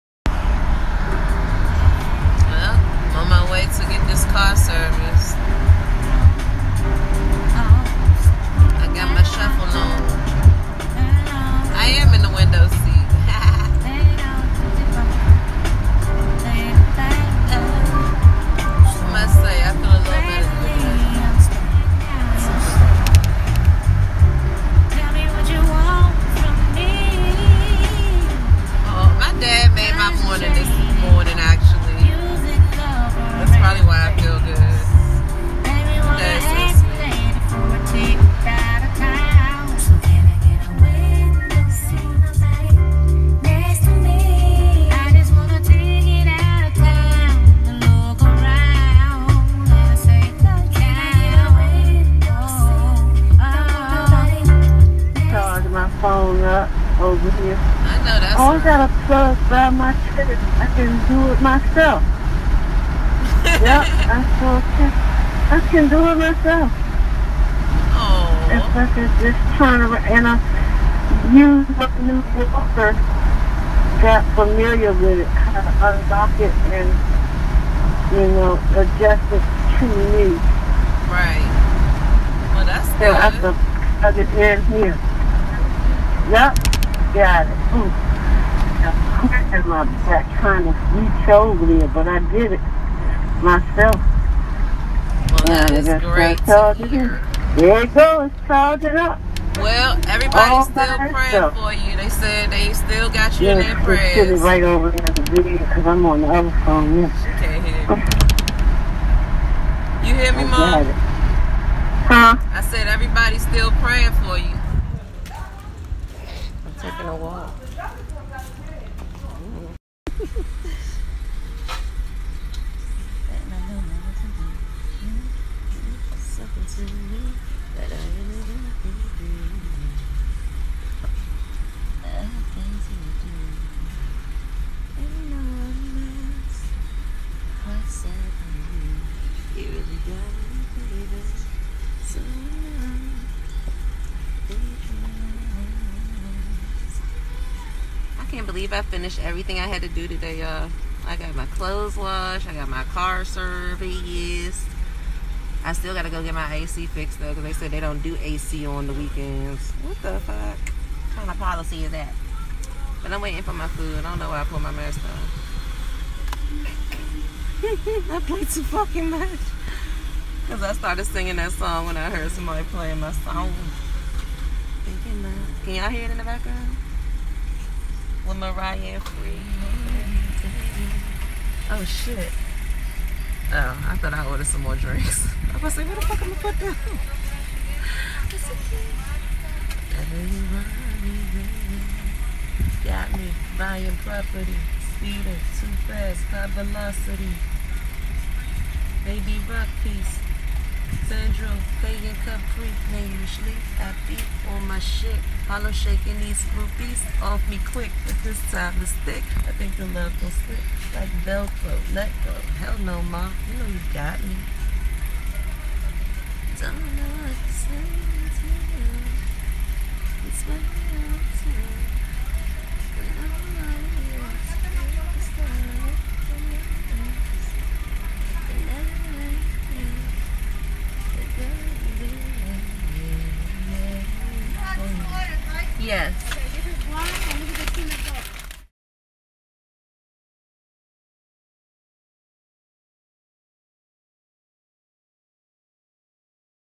Laundry Day + Oil change +Errands ✔️Episode 515- Vlogging with Lady Cee
Check out this episode of vlogging with Lady Cee as I run a few errands for me and my mom.